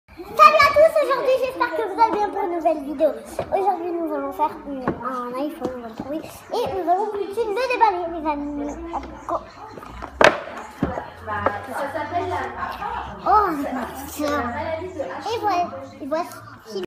Calice et athéna : Clémence 5 ans et sa petite sœur louise 10mois
La petite clémence se lance et s'entraîne pour sa future chaîne YouTube !!! Et invite malgré elle sa petite sœur